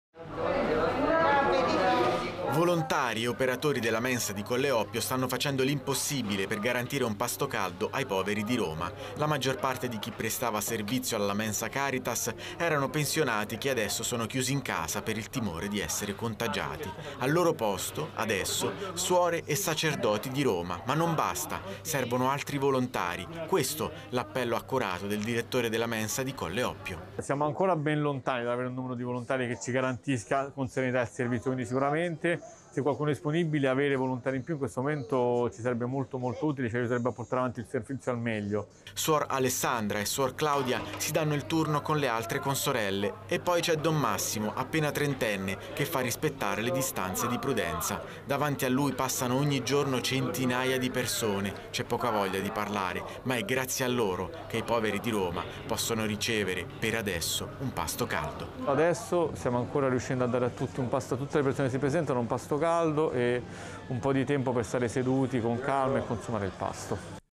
Coronavirus: e in mensa vanno preti e suore
La Mensa "Giovanni Paolo II" a Colle Oppio continua a lavorare a pieno regime grazie anche all'impegno di sacerdoti e religiose. Dal 24 marzo la mensa sarà aperta anche in orario serale venire incontro alle esigenze di quanti non riescono a ricevere altre forme di aiuto